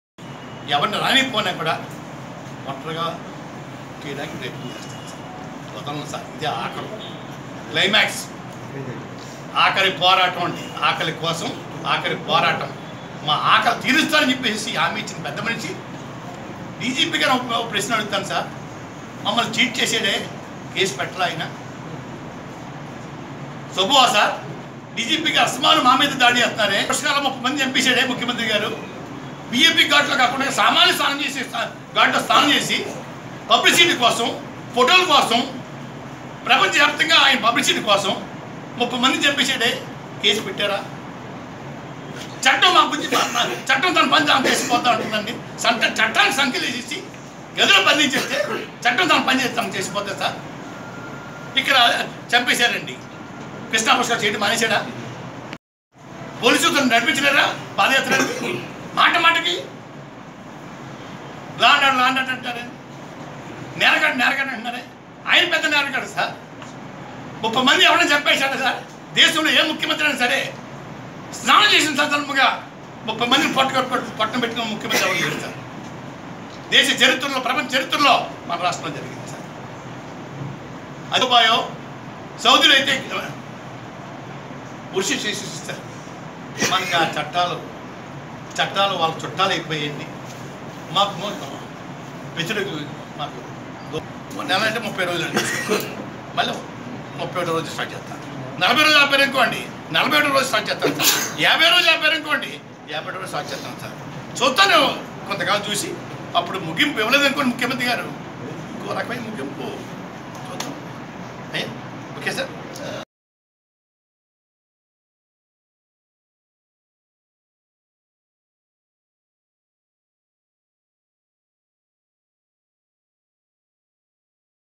ఇది క్లైమాక్స్.. ఆఖరి పోరాటం ఆపమన్న ముద్రగడ
ఎన్ని ఆటంకాలు సృష్టించినా  పాదయాత్రను అడ్డుకోలేరు. నాడు కేసులు లేవని.. నేడు పాదయాత్ర ముందు మల్లీ  ఎలా కేసులగురించి మాట్లాడుతున్నారు. ఇది క్లైమాక్స్. ఈ ఆకారిపోరాటాన్ని ఆపేదిలేదన్న ముద్రగడ. పుష్కరాల్లో ప్రచారంకోసం 30 మందిని చంపిన చంద్రబాబుపై ఎందుకు కేసునమోదుచేయలేదన్న ముద్రగడ..